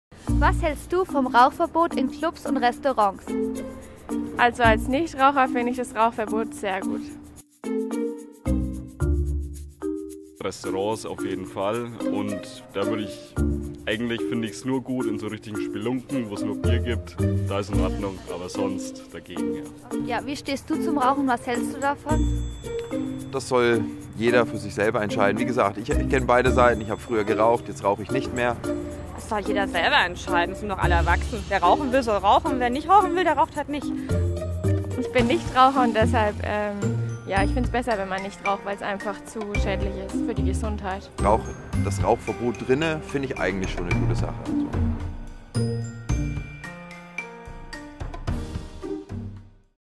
10 Jahre Rauchverbot in Clubs und Bars
Draußen zittern statt drinnen qualmen: Findest Du es gut, dass man seit mittlerweile knapp 10 Jahren nur noch vor der Türe rauchen darf?

Rauchfreie Parties: Schweinfurt, was geht? I